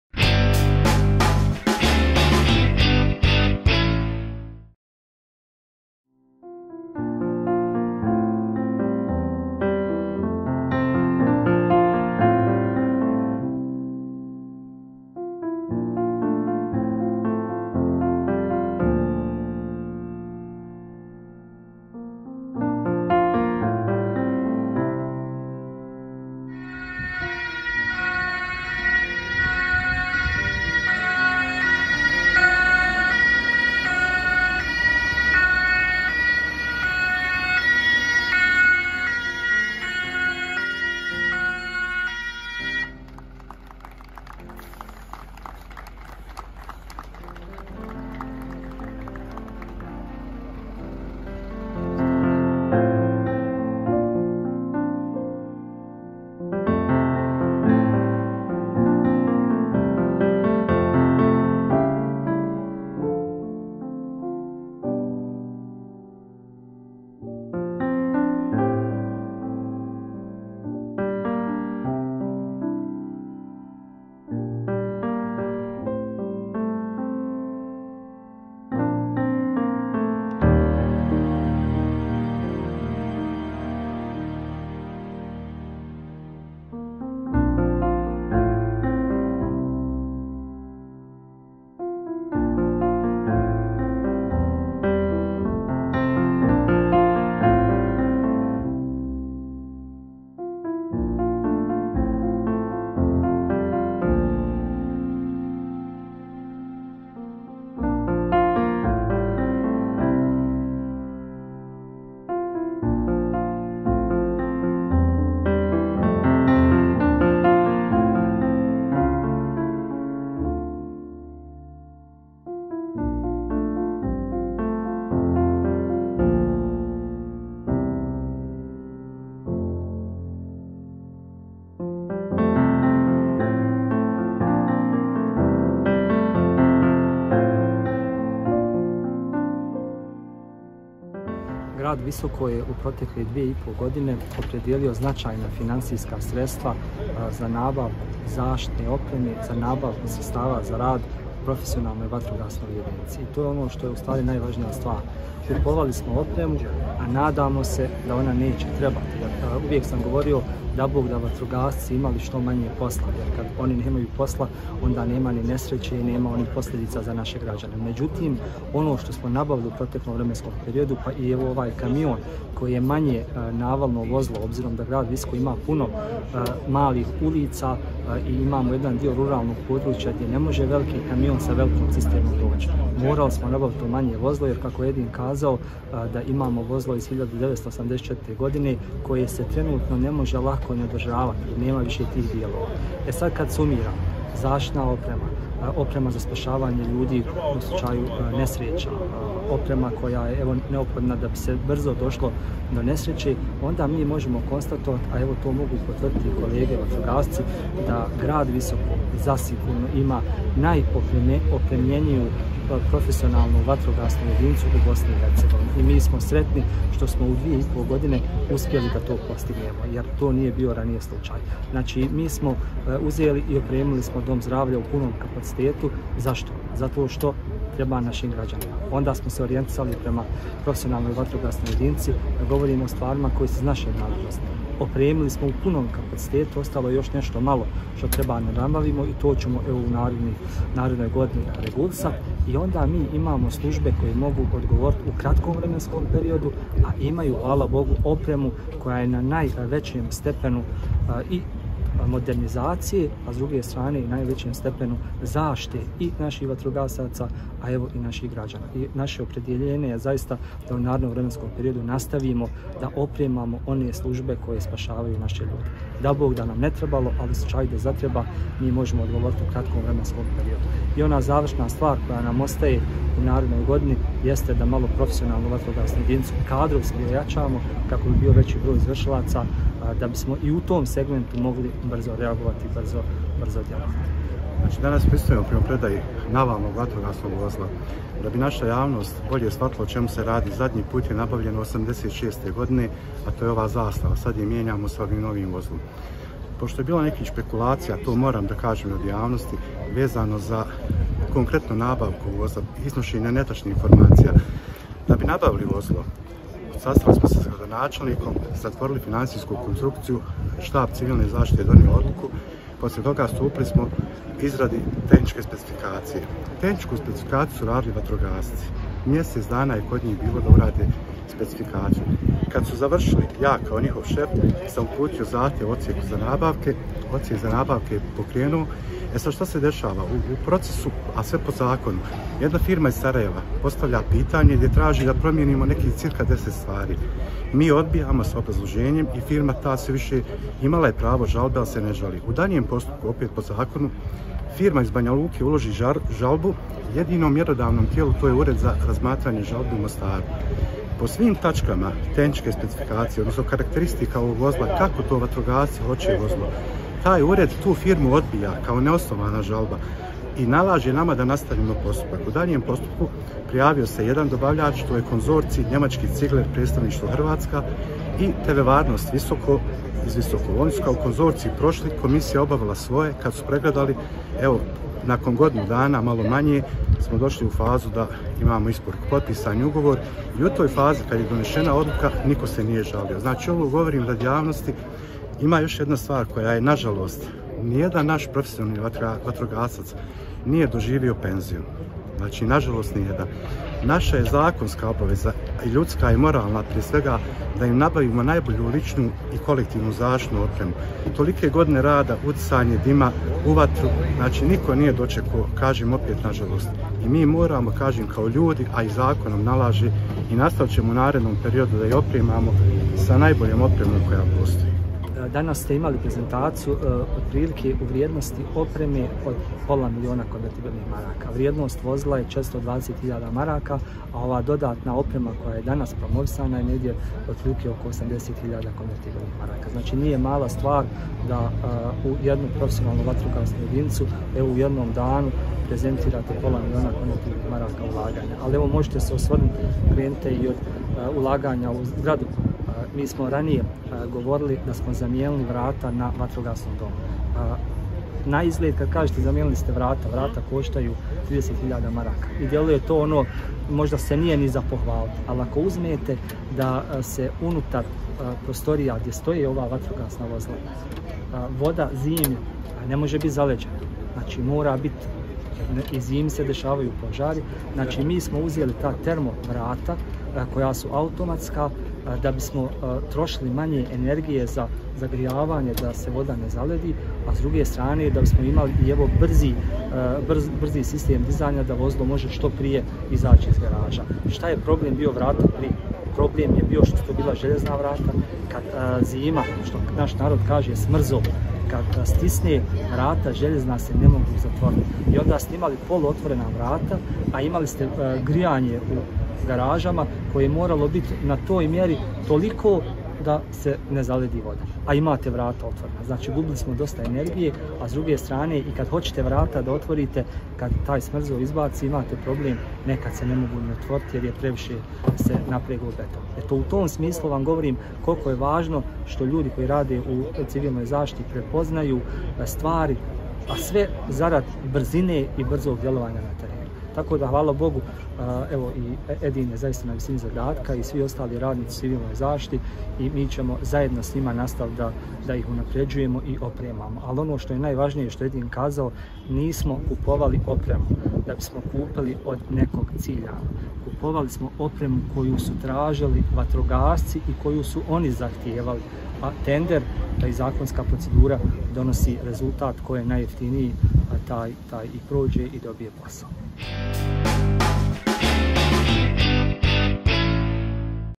Predstavljeno novo navalno vozilo za visočke vatrogasce (22.09.2023)
U petak, 22. septembra 2023. godine, ispred Vatrogasnog doma u Visokom upriličena je prezentacija novog vatrogasnog vozila koje će postati dio Profesionalne vatrogasne jedinice Visoko. U pitanju je manje navalno vozilo koje je bilo neophodno za rad i brzo djelovanje visočkih vatrogasaca, posebno u uskim gradskim ulicama, te u ruralnim područjima do kojih nisu mogla dolaziti velika vatrogasna vozila.